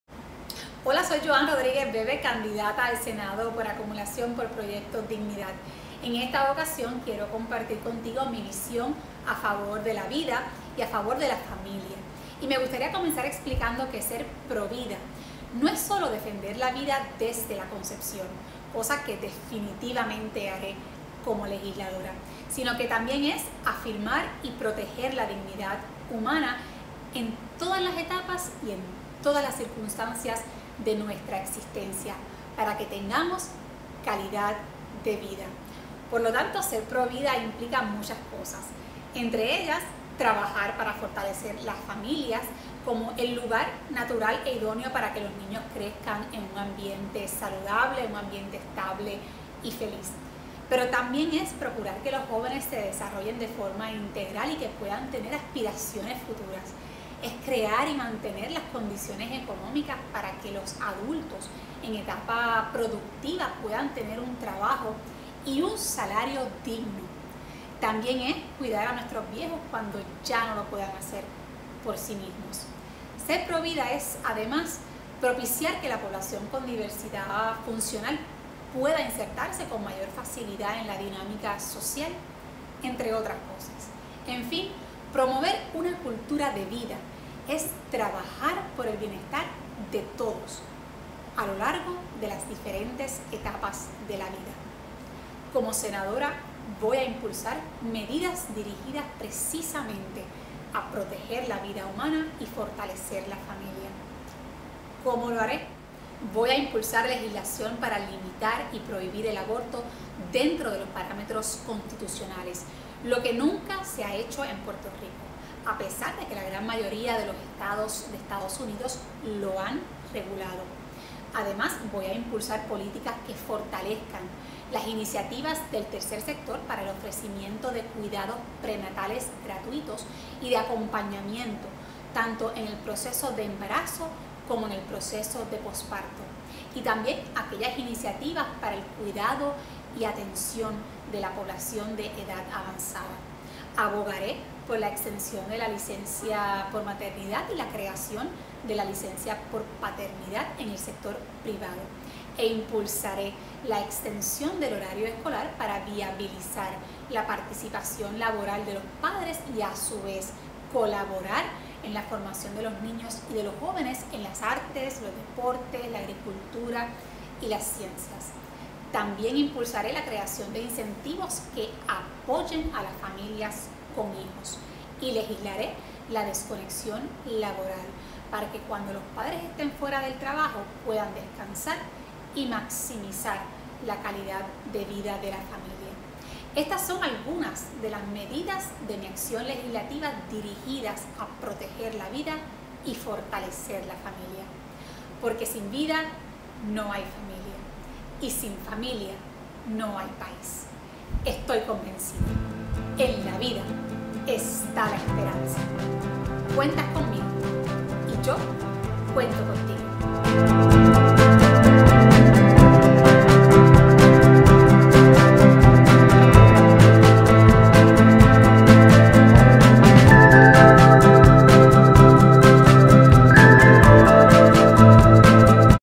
PRO VIDA y FAMILIA
Como senadora, impulsaré medidas dirigidas a proteger la VIDA humana y a fortalecer la FAMILIA, porque sin vida no hay familia y sin familias no hay País. Estoy convencida: ¡en la vida está la esperanza!